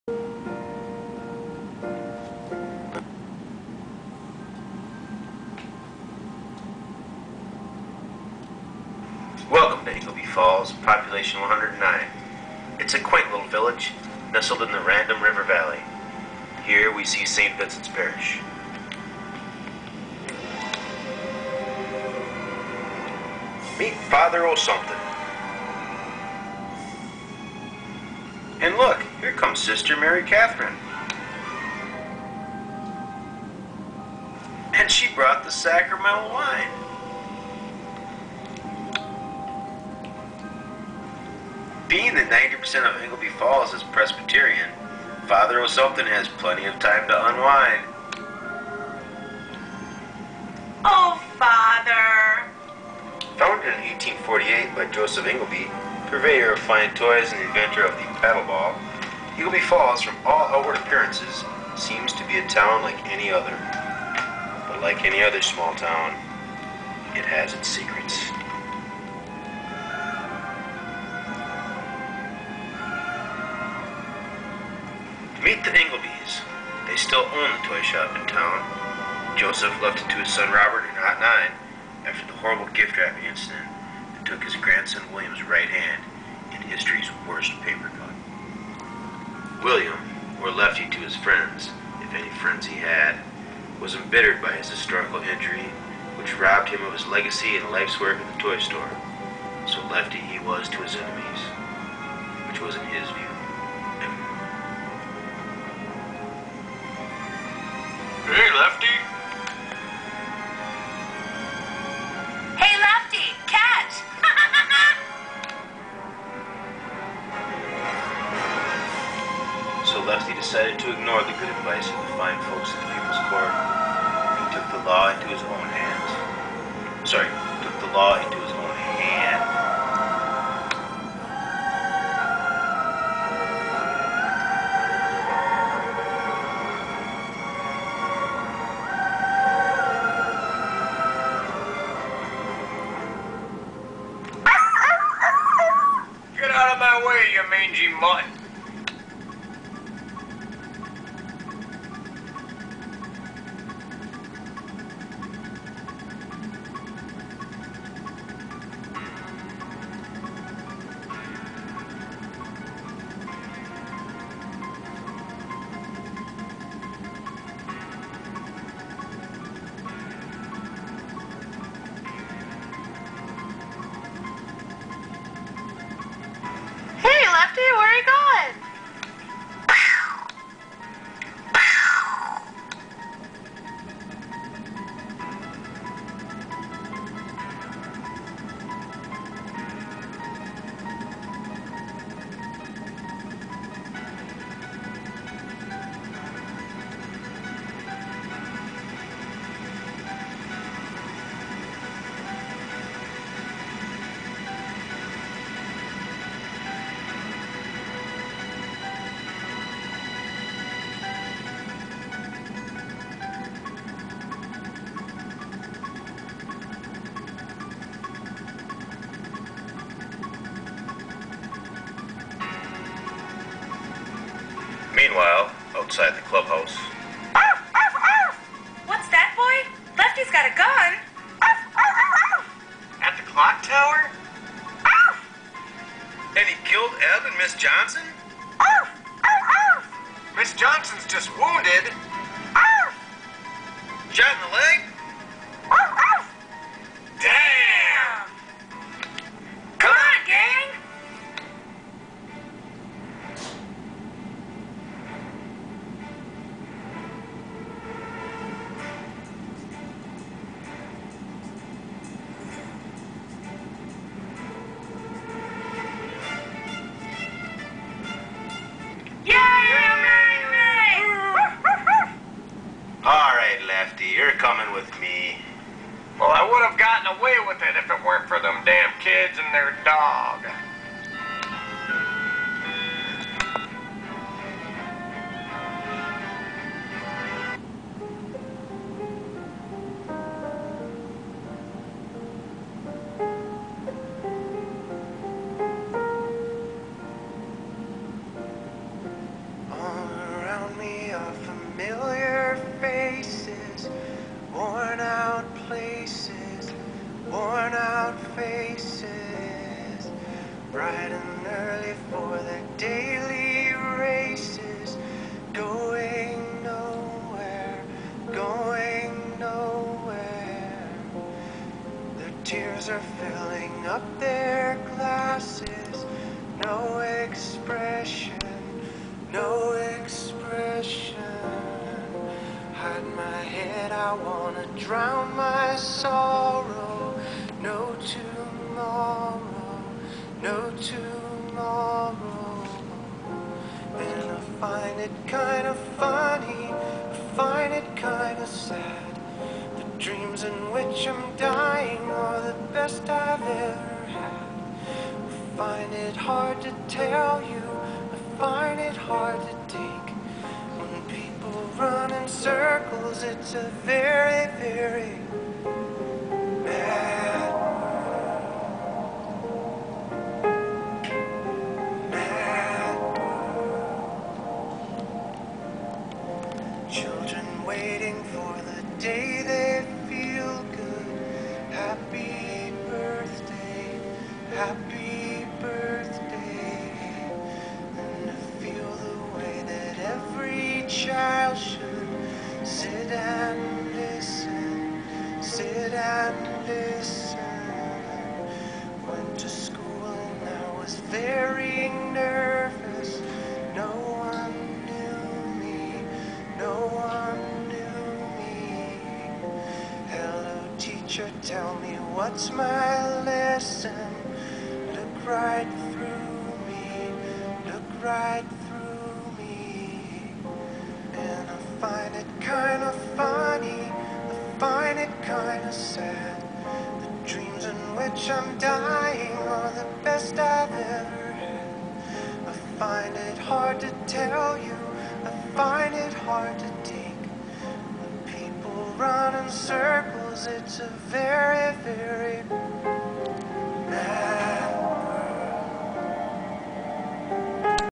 The Terror in Ingleby Falls
A disturbing look into the evil that lurks in the heart of every christmas village